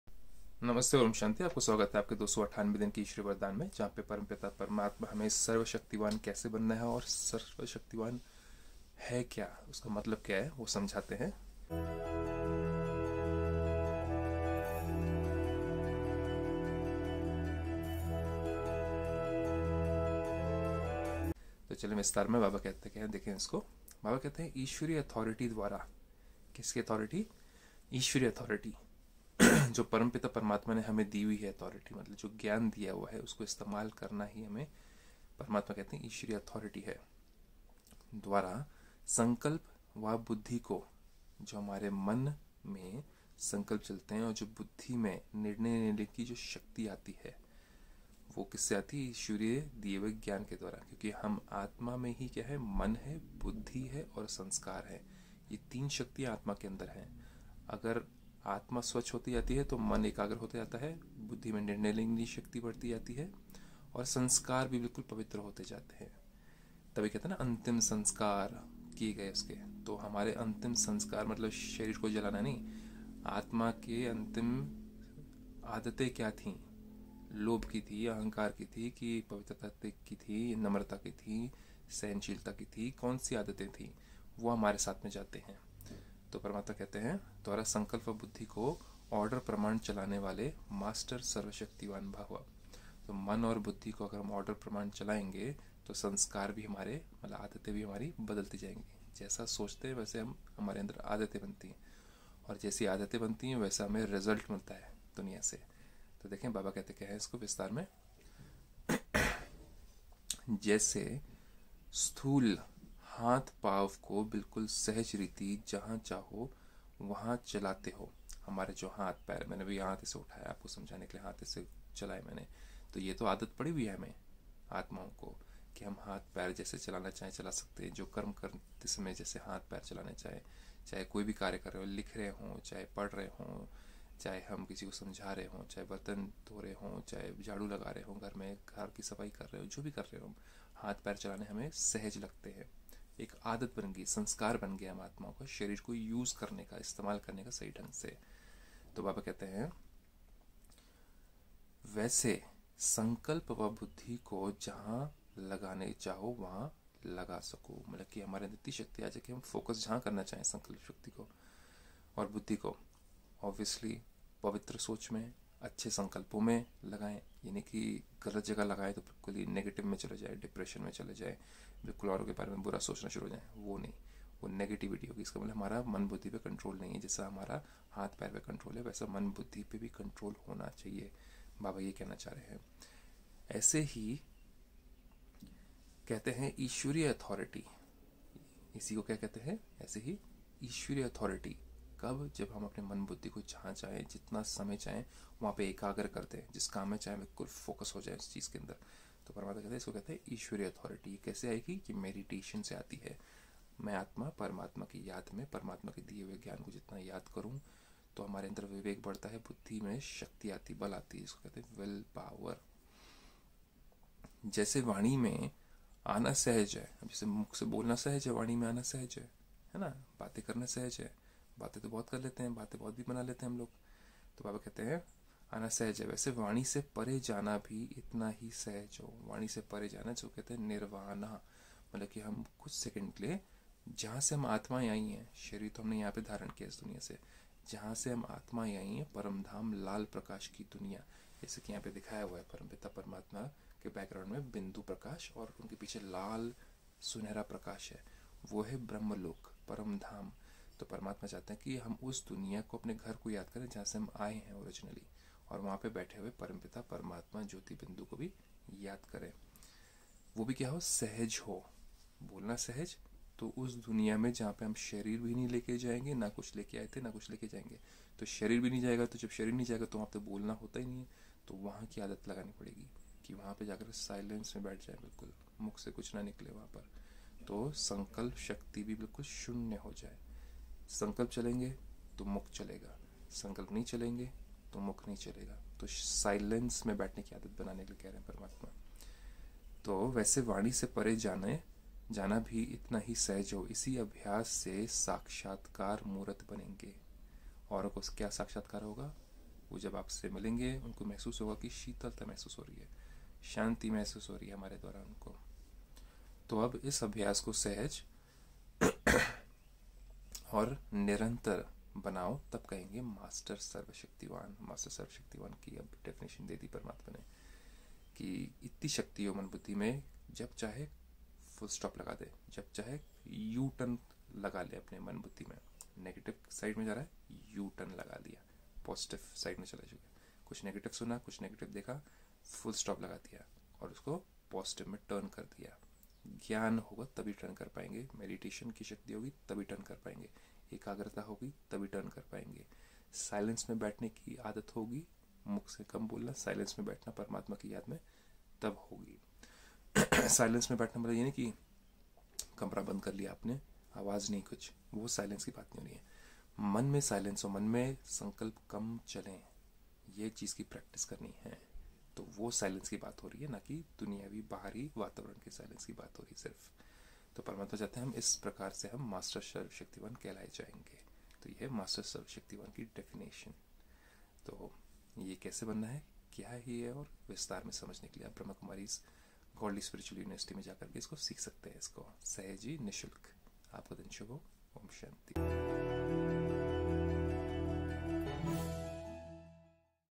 दैनिक वरदान |298|हमारे दिमाग और बुद्धि को क्रम में रखनाएक मास्टर सर्वशक्तिमान प्राधिकरण की तरह
मन और बुद्धि को वश में करने का एक ही उपाय है। मास्टर सर्वशक्तिमान बनने का एकमात्र तरीका। आइए आज यहां इसके बारे में जानें।

ओम शांति
दिव्य प्रेम परम